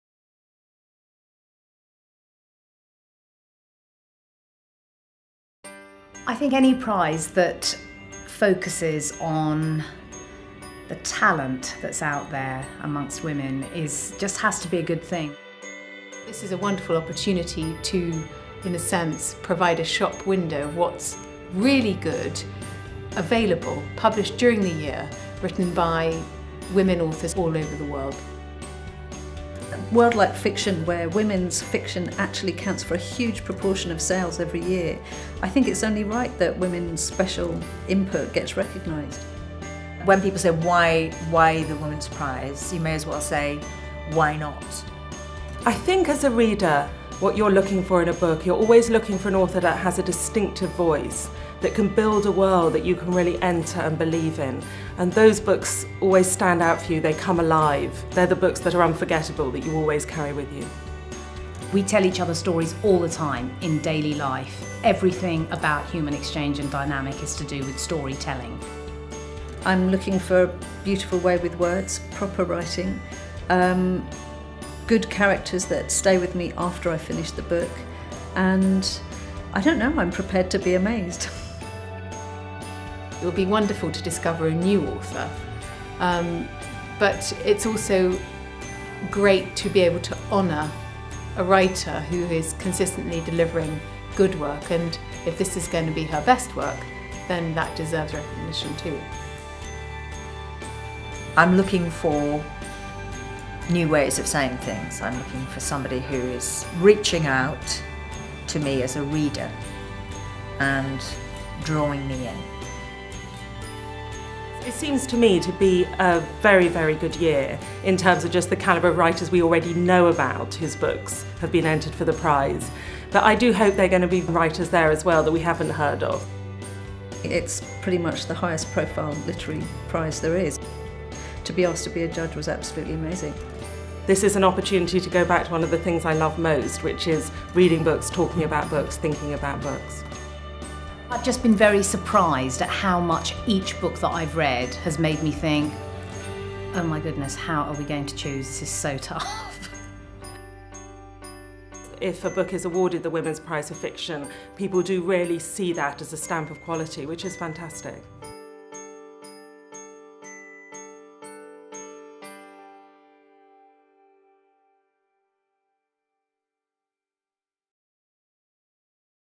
Women's Prize judges interview.mp4
The Women's Prize for Fiction judges share what they're looking forward to most about judging this year's Prize.